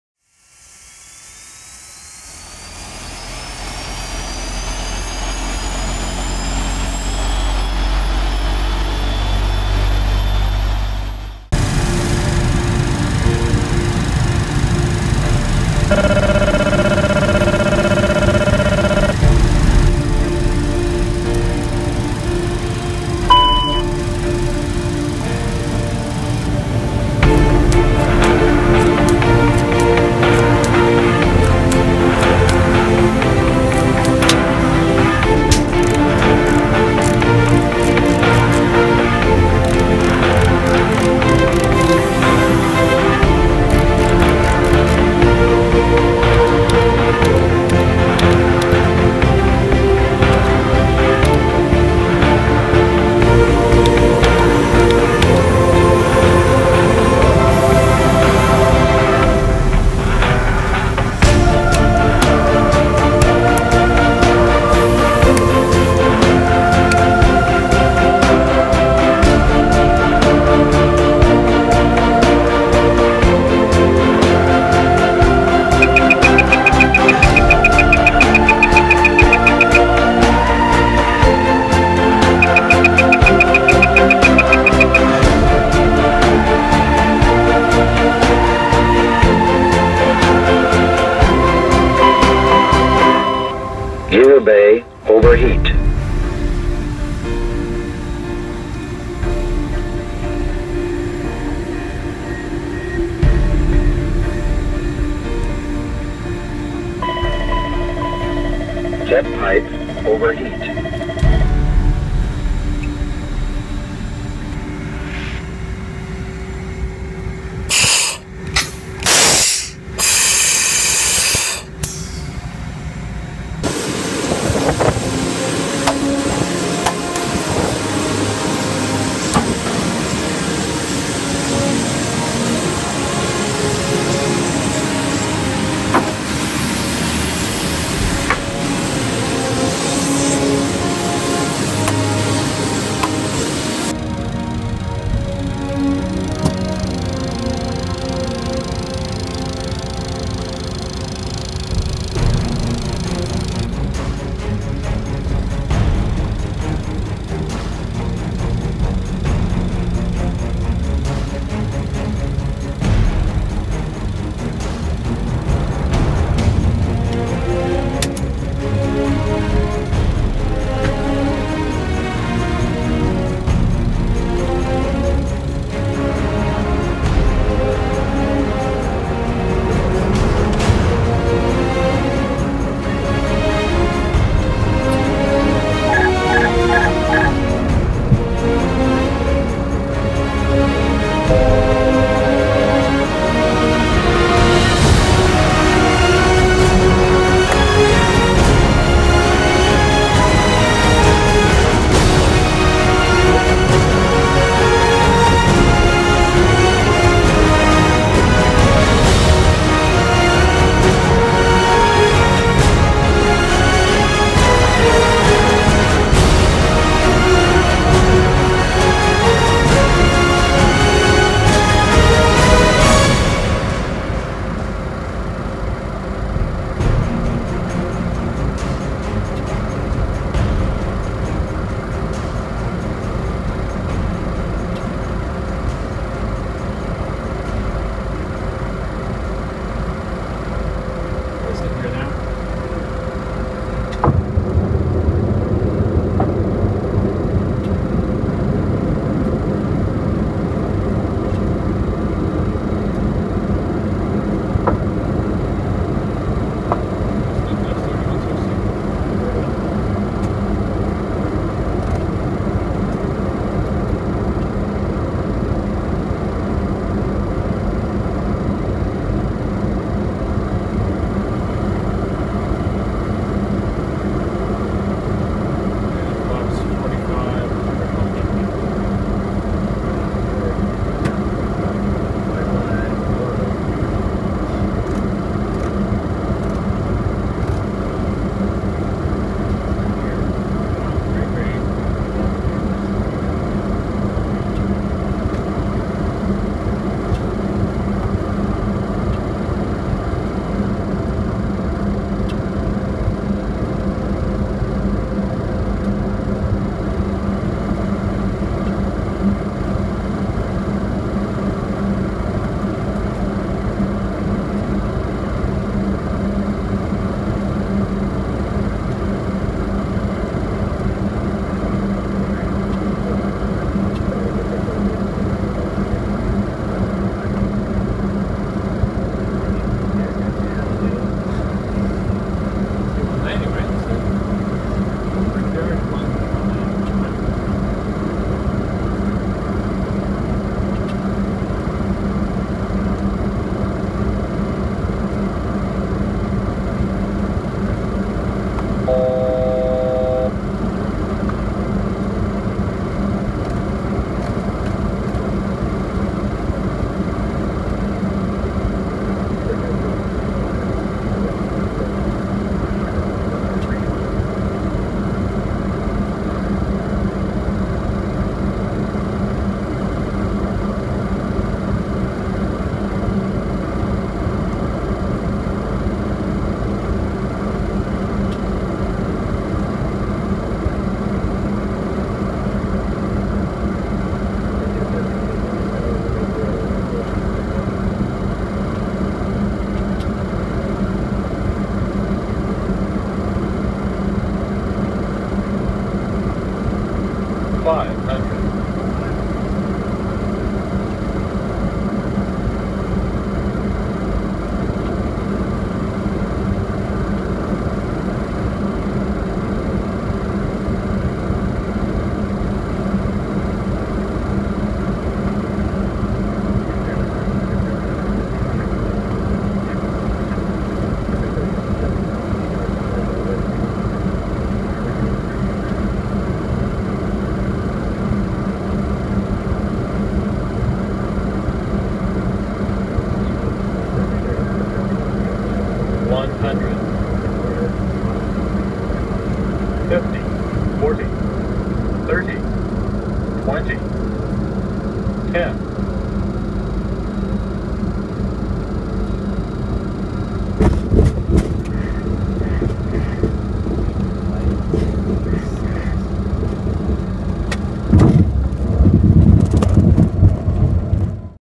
CRJ sounds will be the best out there
Working to provide the best and most realistic sound to flight simulations with help of my buddy pilots. You WILL get the ultimate sound package! Crjay all the way!